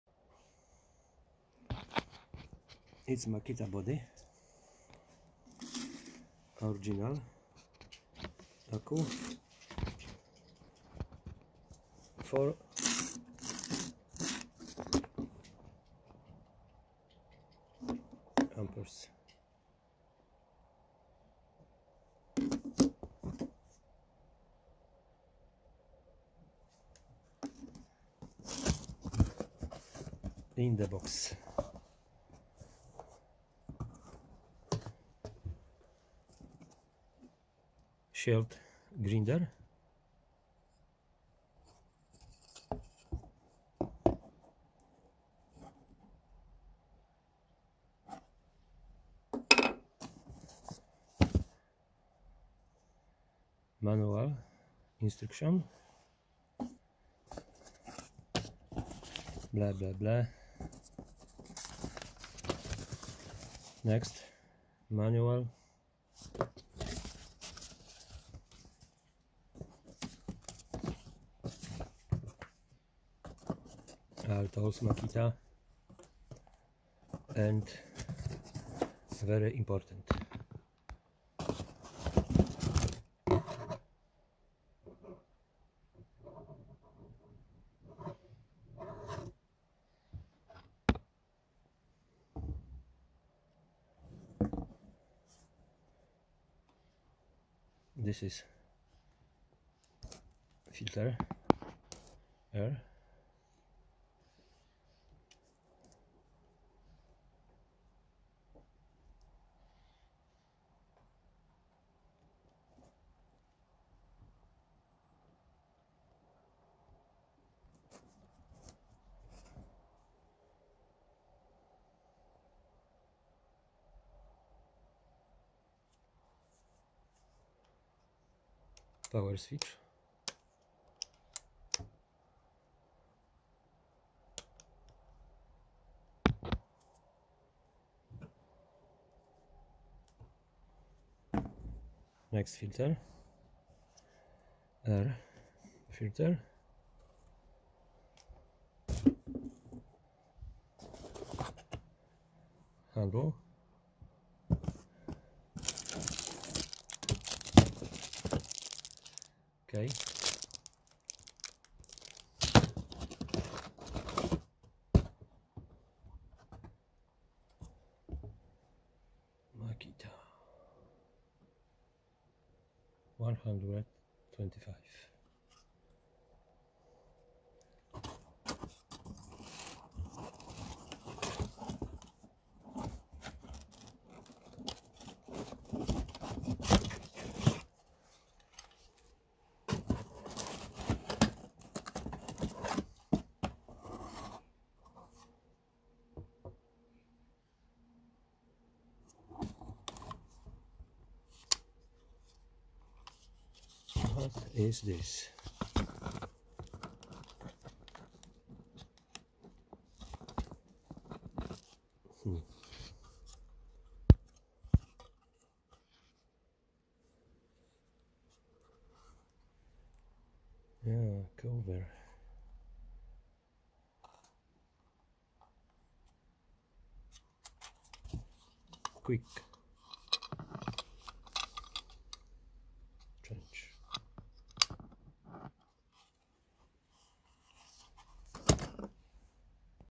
Unboxing Makita DGZ 511
My first battery-operated angle grinder.  Sometimes the situation requires it.  It is not possible to have power everywhere

Moja pierwsza szlifierka kątowa na baterie. Czasem tego wymaga sytuacja. Nie wszędzie można mieć zasilanie.